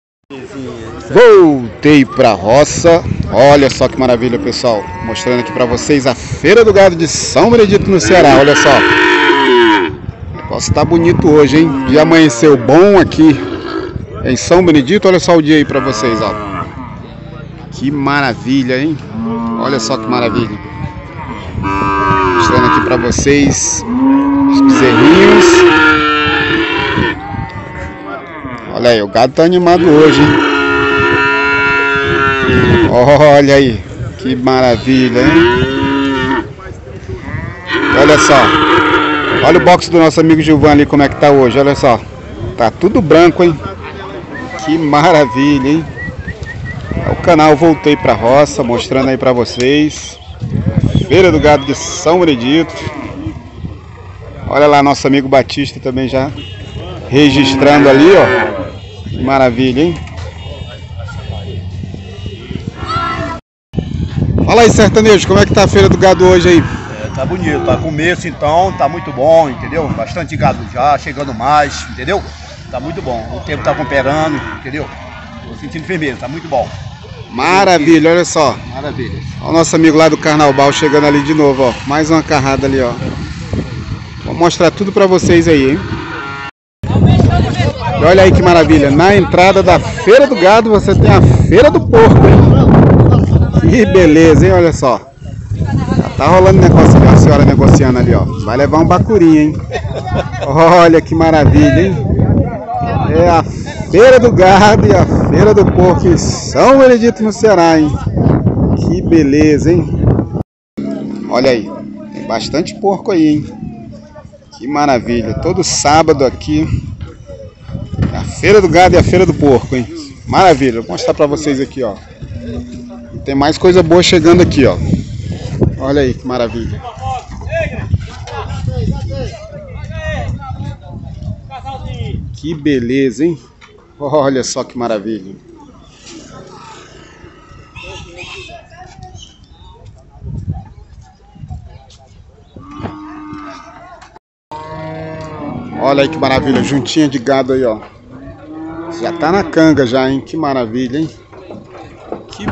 Canal Voltei pra Roça Cortes da Feira do gado de São Benedito #001
Veja os cortes da Feira do Gado de São Benedito e curta a nossa aventura pela cidade de São Benedito na Serra da Ibiapaba - Ceará. Se gostar dos nossos vídeos não deixe de curtir, comentar e compartilha, Voltei Pra Roça!